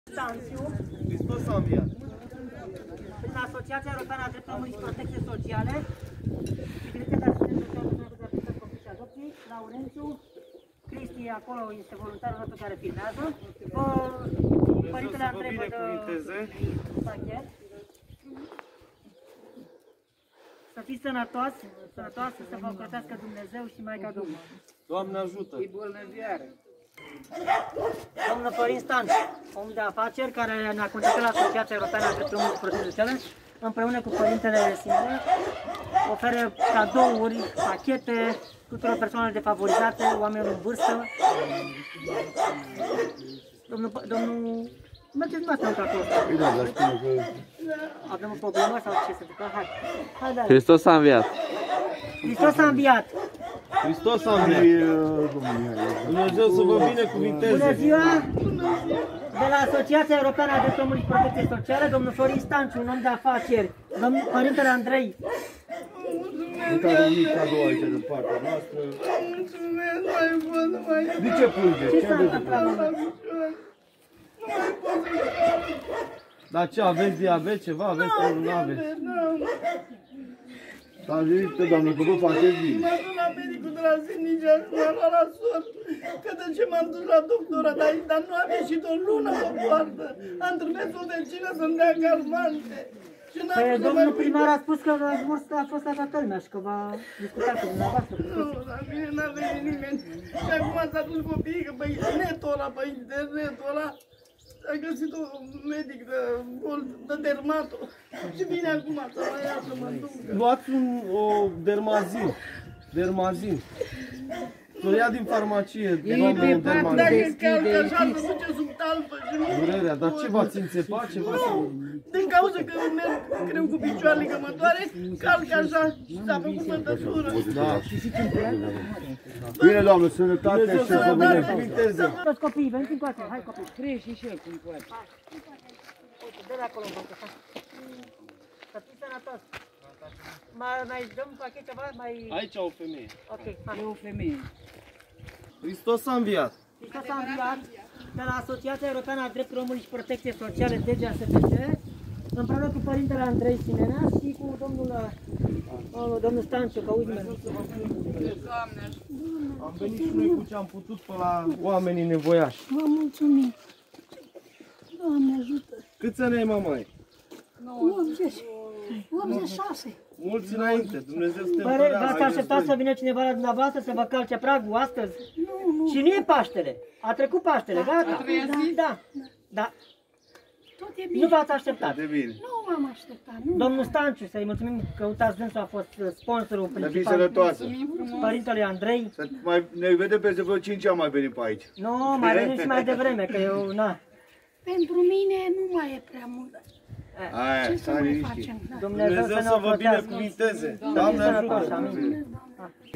Mihai Căldăraru Director Executiv - Gest frumos persoanele defavorizate din Comuna Pietroșani!
Cu ajutorul lui Dumnezeu cu binevantarea Preasfintitului Galaction,Episcopul Alexandriei si Teleormanului,Parintelui Simene Andrei, Dl Stanciu Florin, Mihai Căldăraru Directorul Direcției Generale de Asistență Socială

Împreună cu echipa de voluntari ai Direcției Generale de Asistență Socială   Muti George Laurențiu,  Mereuță Petruț Cristian.

Au adus zâmbete pe chipul persoanelor defavorizate și bătrâni neajutorați de propii copiii din Comuna Pietroșani județul Teleorman.